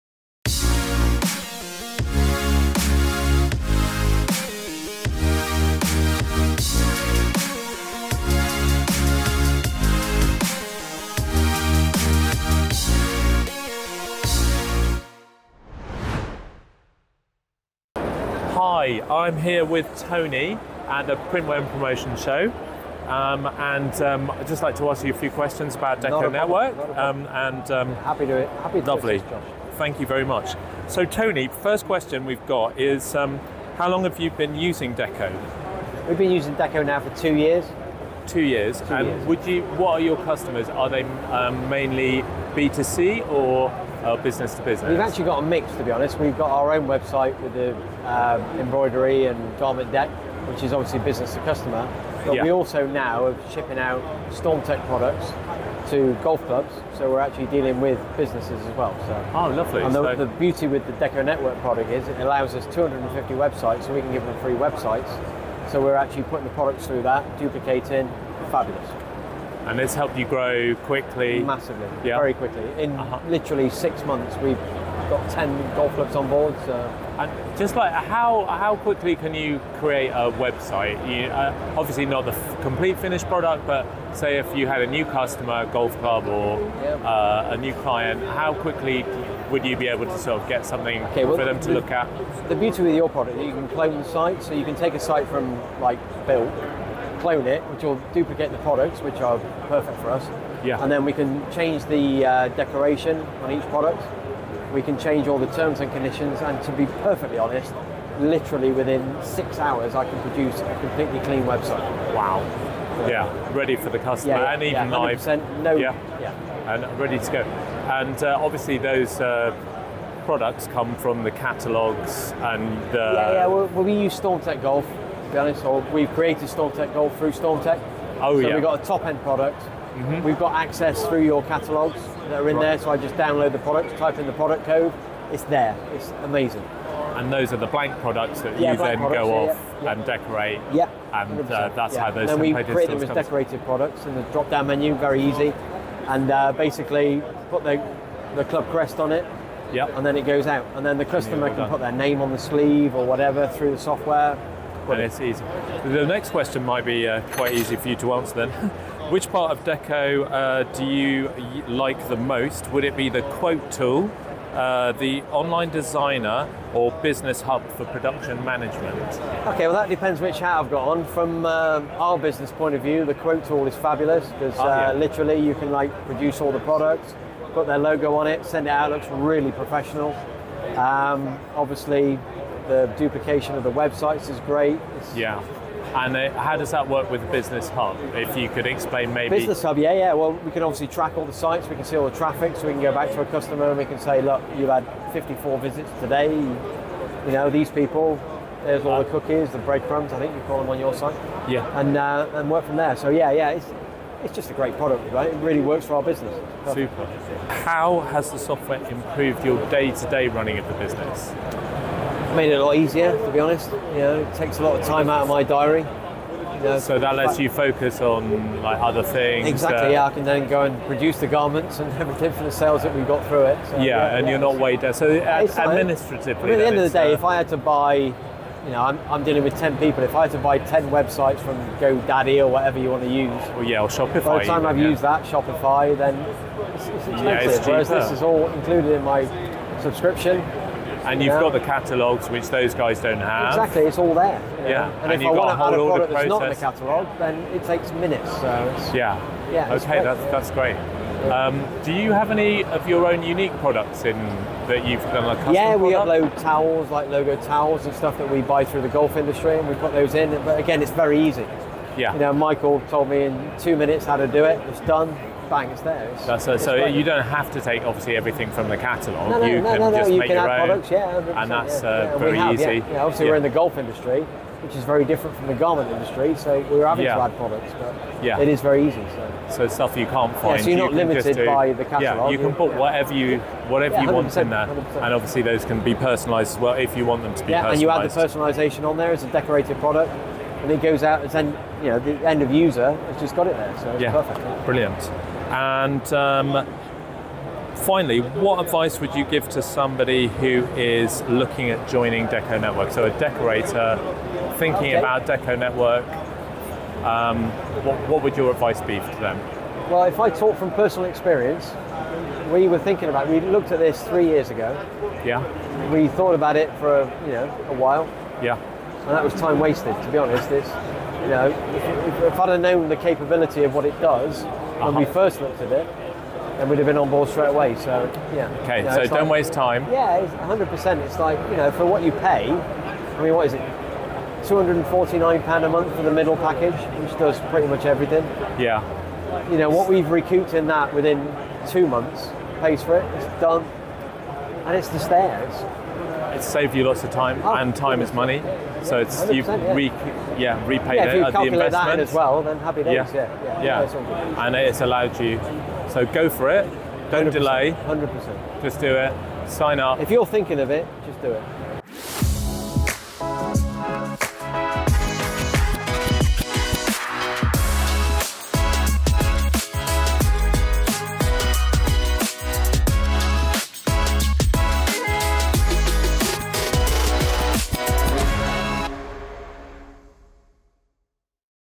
Why All Stitched Up Chose DecoNetwork
Tony Mealing took his passion of golf and parlayed it into a successful custom apparel business in Bristol, United Kingdom.

With the help of DecoNetwork, Mealing has taken his custom apparel business to the next level and credits our All-in-One software as the reason why.